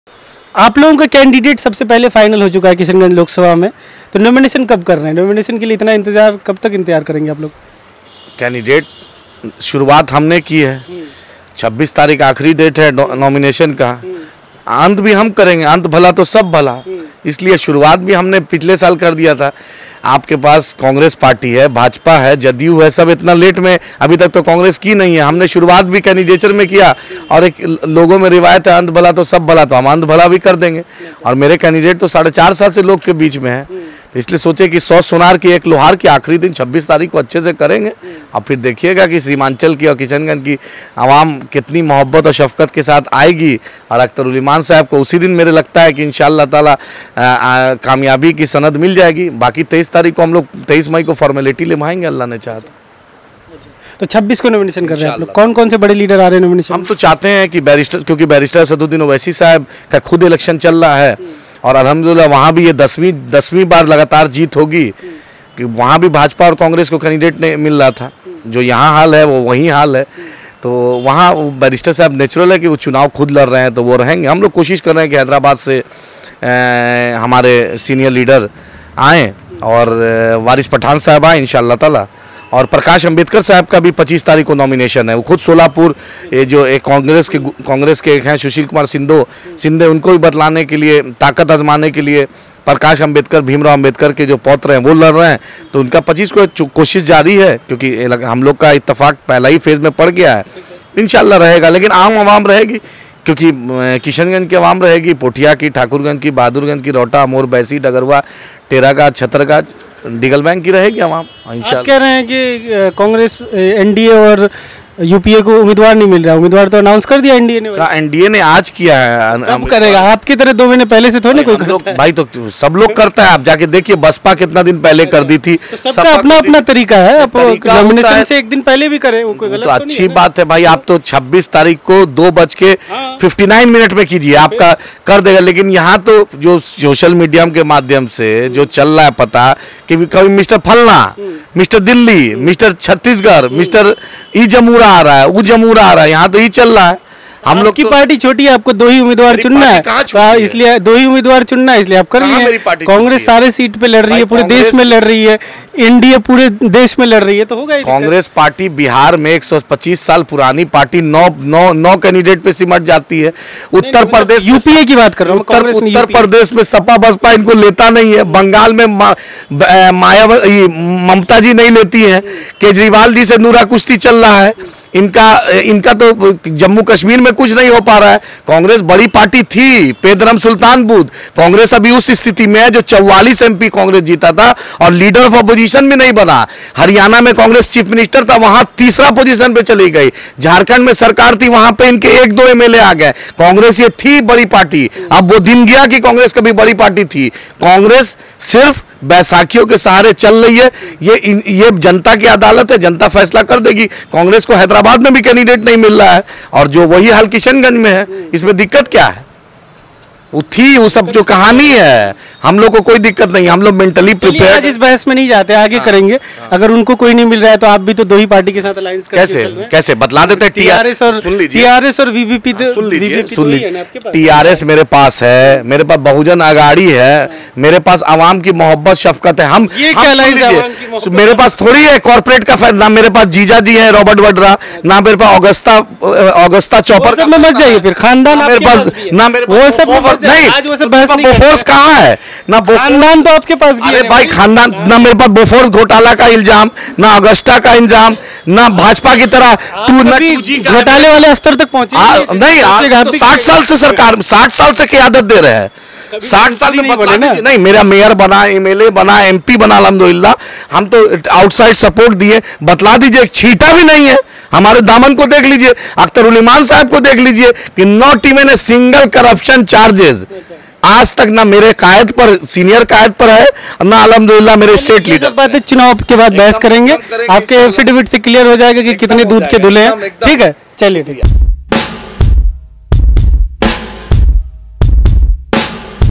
किशनगंज: महीनों पहले प्रत्याशी घोषित होने के बाद भी AIMIM ने क्यों नहीं किया है नामांकन
किशनगंज: महीनों पहले प्रत्याशी घोषित होने के बाद भी AIMIM क्यों कर रही है नामांकन में देरी?